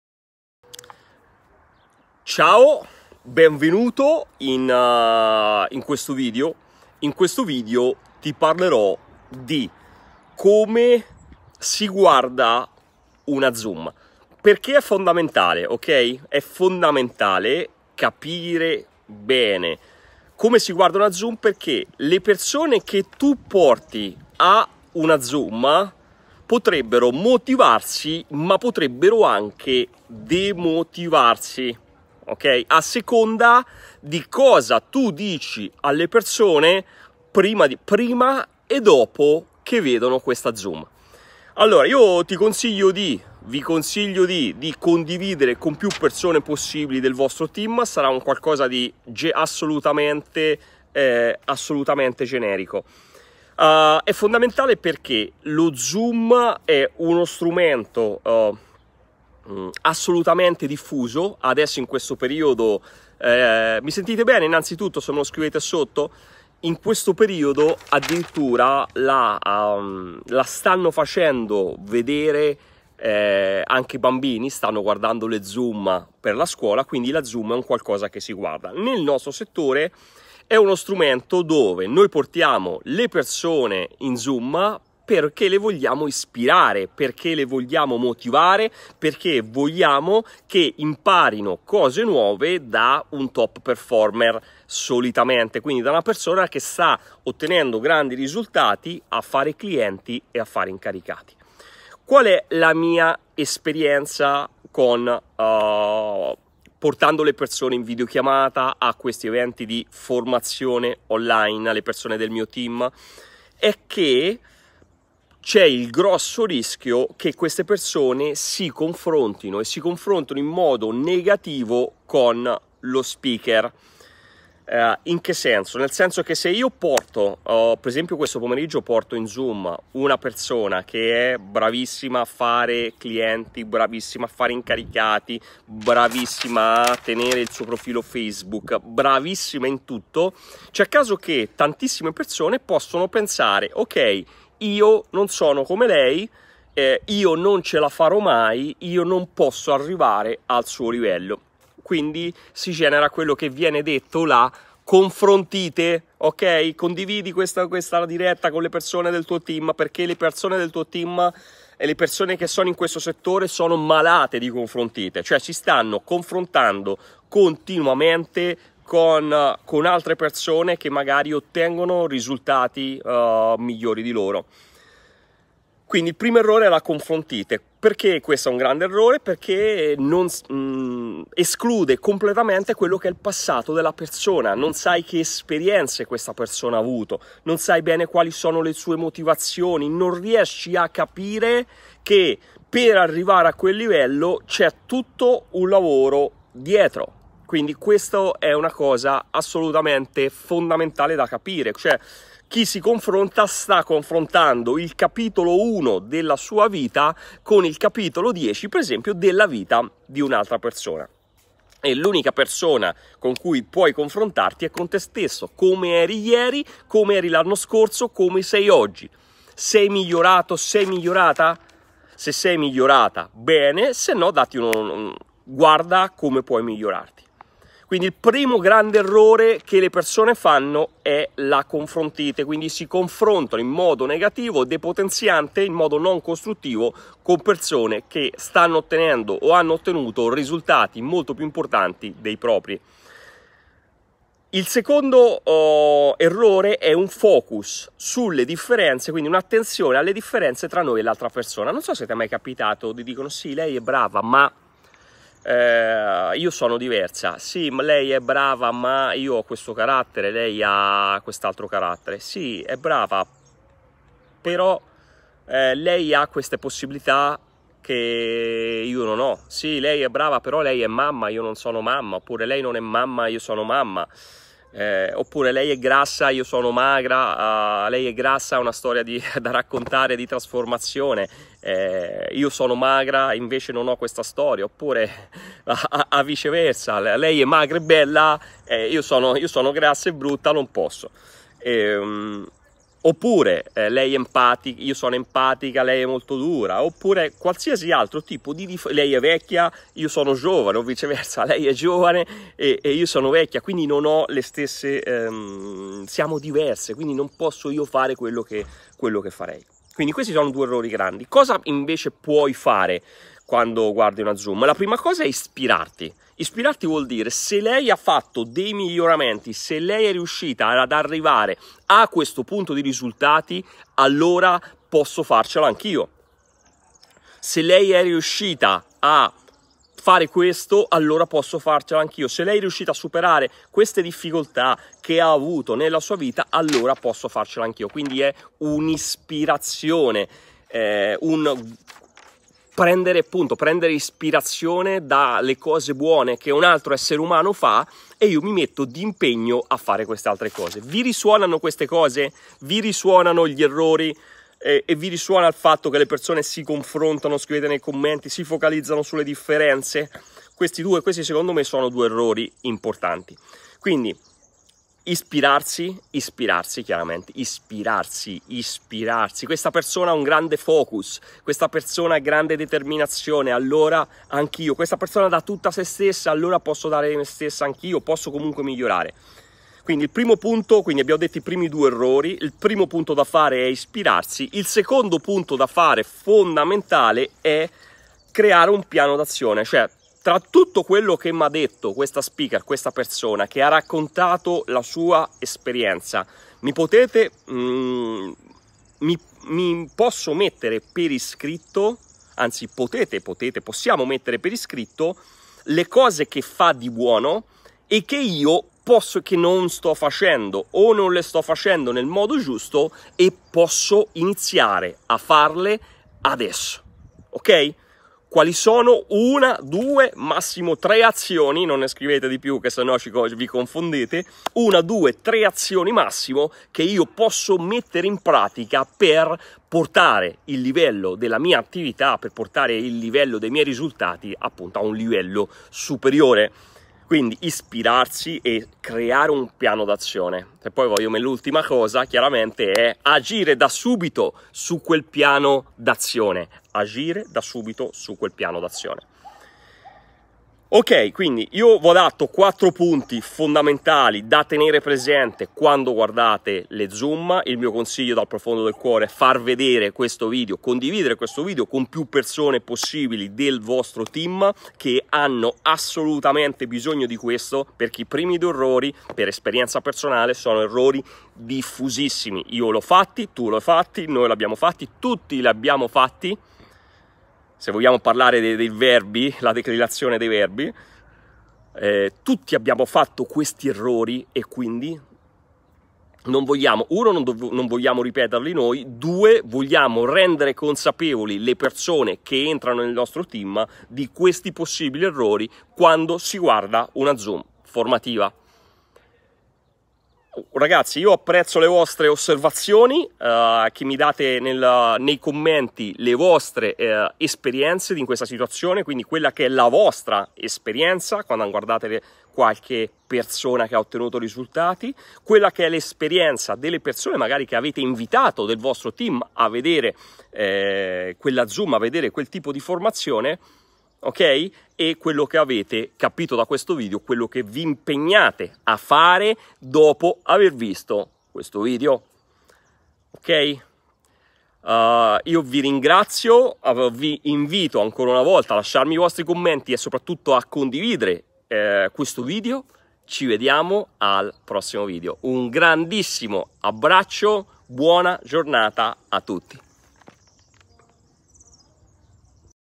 Gli errori che si fanno quando si guarda una zoom network marketing
In questo video ti parlo dei principali errori che le persone fanno quando guardano una zoom con un leader o un top performer nel settore del network marketing.
L’errore principale è la confrontite e guardare le cose in cui siamo diversi, il tutto in modo altamente depotenziante.
Nel video ti parlo anche di cosa puoi fare per imparare al meglio